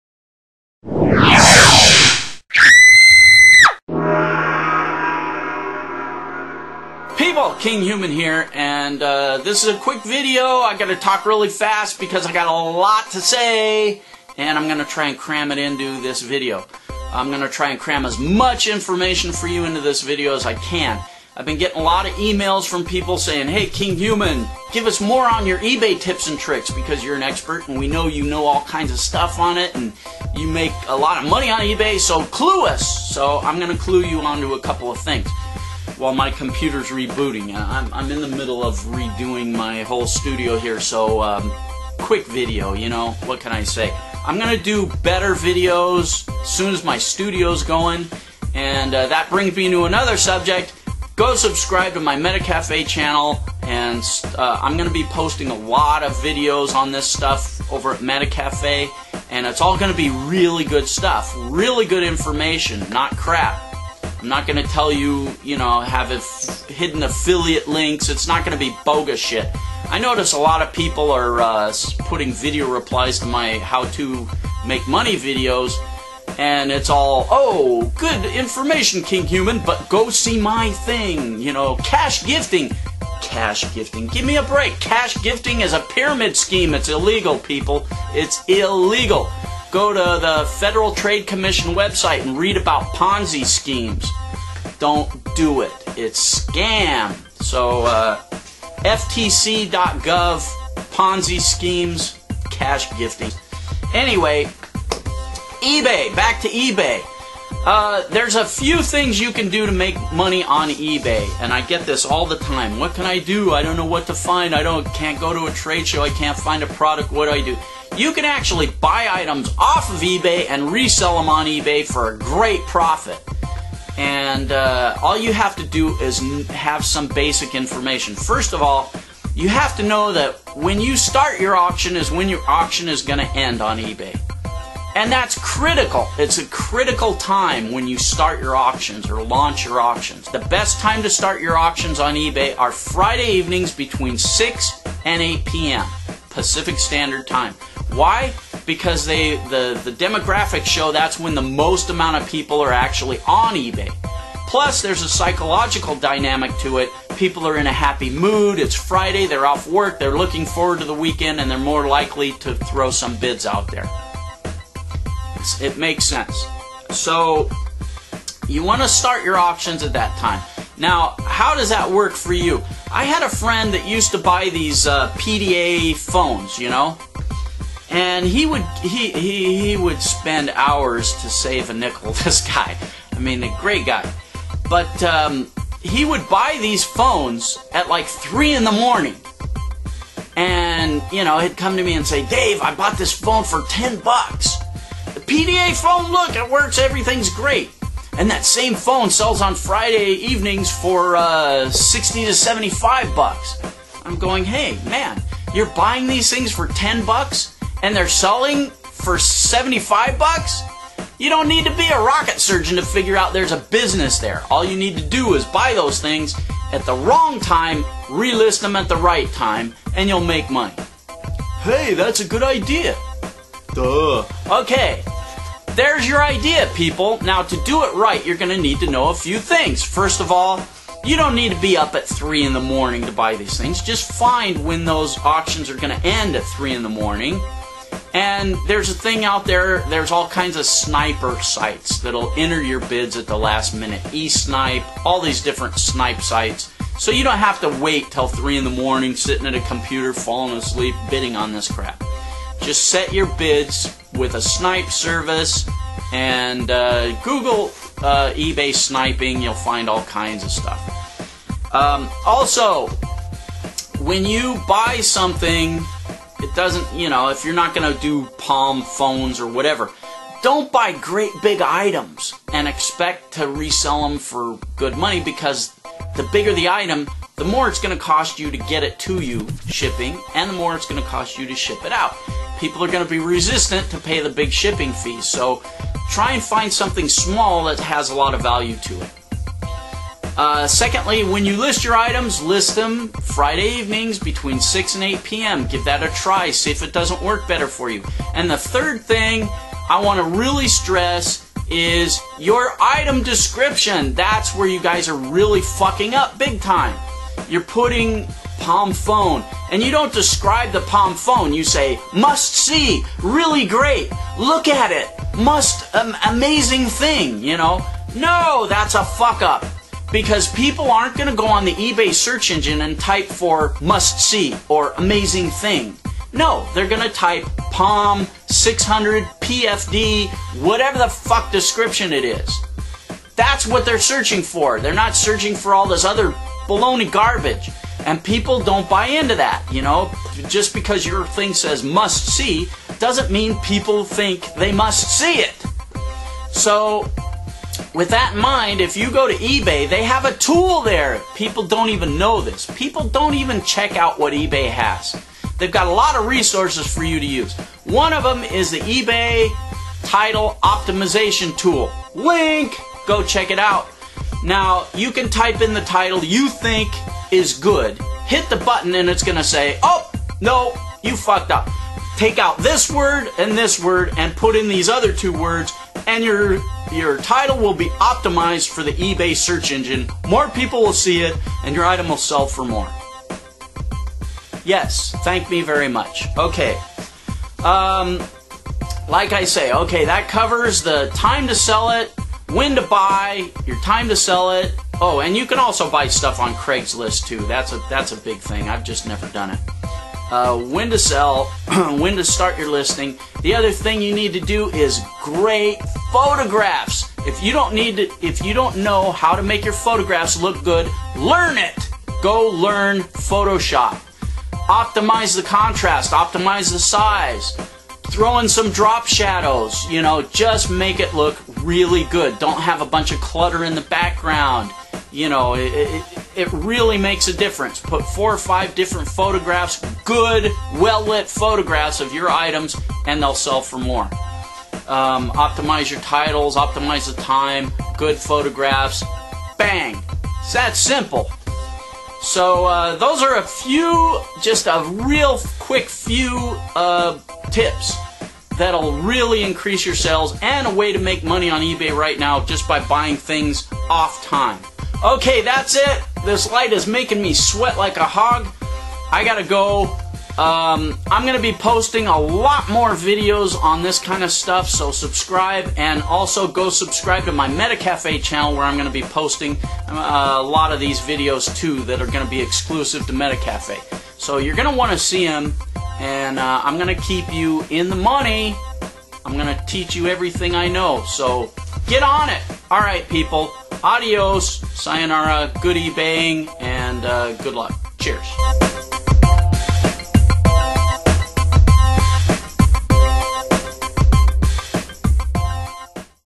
Make An Extra $30K a Year / Ebay Secret
READ THIS!...
Here is the link to my Metacafe channel.

Here is the link to the Ebay title optimization tool:

Now when trying to buy items on Ebay for resale, don't think you need to buy the Palm phones I spoke of in the video.

Search Ebay for "What's Hot" and find something that isn't listed well.  Meaning, the auction has a bad title, it's set to close at an odd hour and it has bad images.

This can be any sort of item, but try to make it an item that's easy to ship & in high demand.

This video is just an intro to these concepts.  I'll post a more detailed video on this subject soon.  Meanwhile, start scanning Ebay and see if you can see auctions of in demand items that are being listed badly.  Then compare that item against the same items that have SOLD.  If there is a big enough difference that you are assured a good profit, CONGRATS!  You have found gold.

Find enough of these nuggets and you can make a great income for yourself.

Cheers!
kh